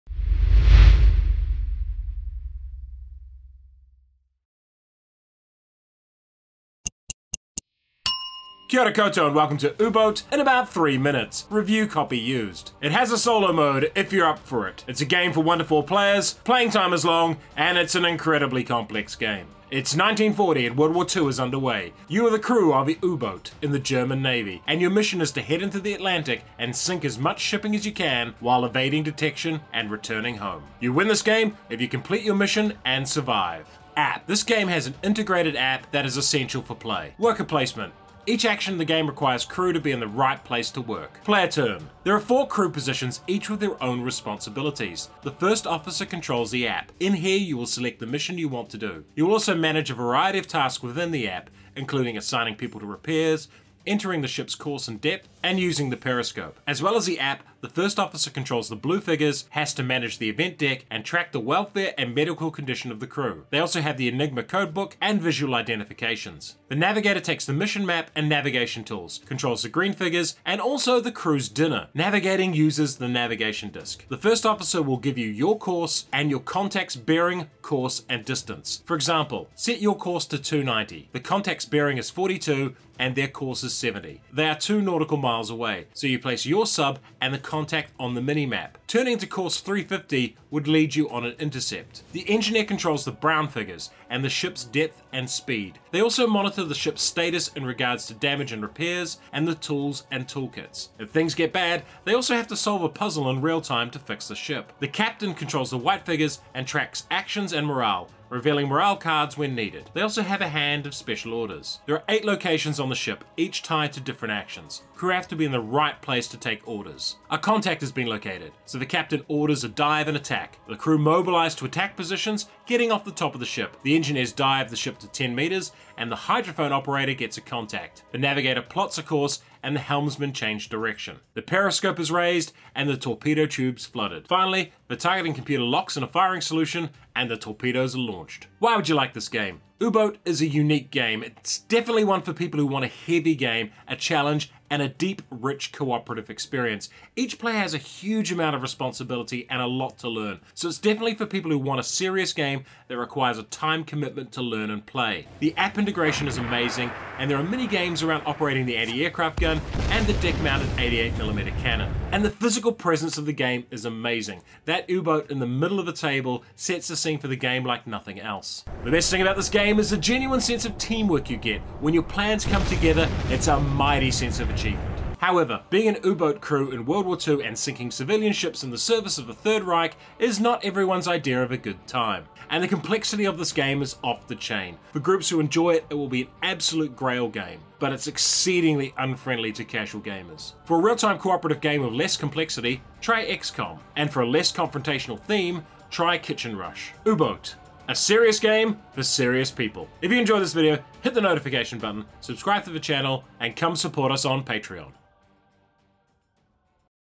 Uboot in about 3 minutes
A short summary of It's a Uboot in about 3 minutes

Designer,Bartosz Pluta, Artur Salwarowski,
Artist,Chris Clor,
Publisher,PHALANX,

3 minute board games are J Carmichael and S Rodgers